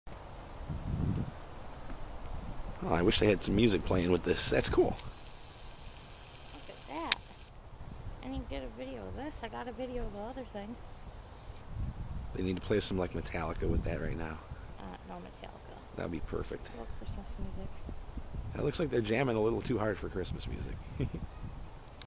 Santa's Rock Band Lilacia Park 1-1-16 Lombard, IL
Santa and his band jam out to some unknown music! They look like they're having a good time!